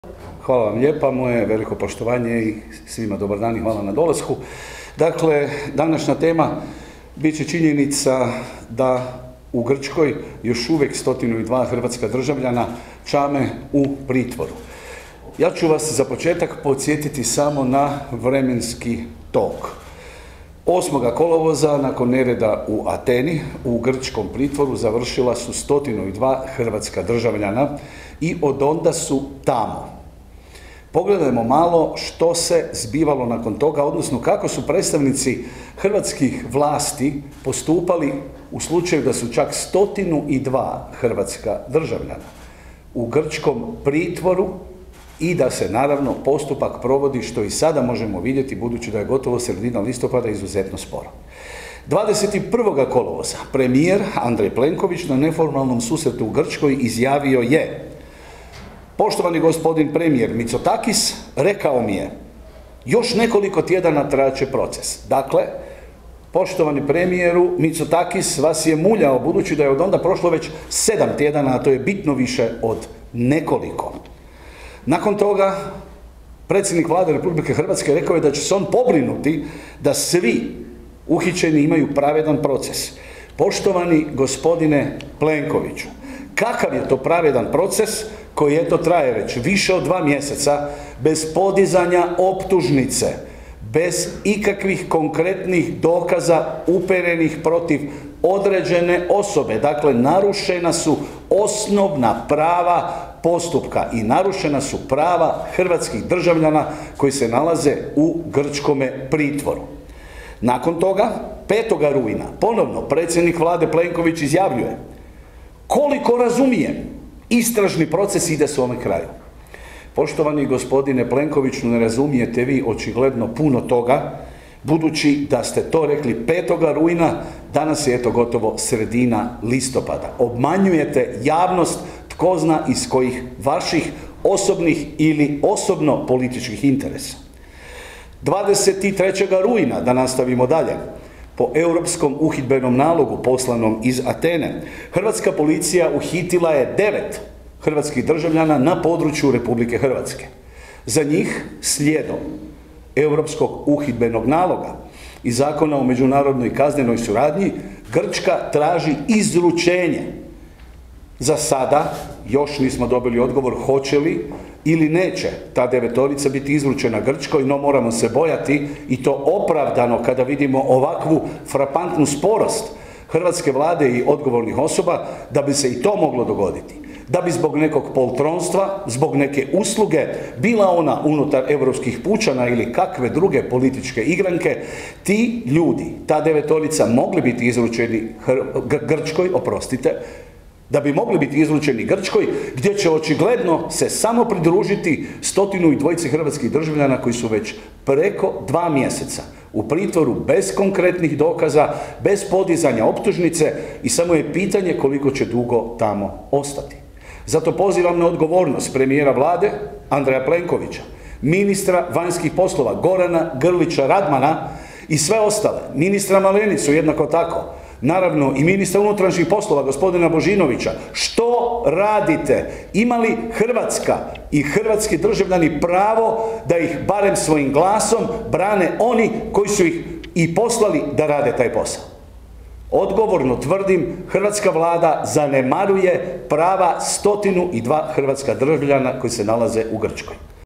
Dretar: "Vlada zanemaruje prava 102 hrvatska državljana koji još uvijek čame u grčkom pritvoru"
Na konferenciji za medije održanoj u Saboru o temi "Uhićeni Hrvati u Grčkoj" govorili su Davor Dretar i Damir Markuš.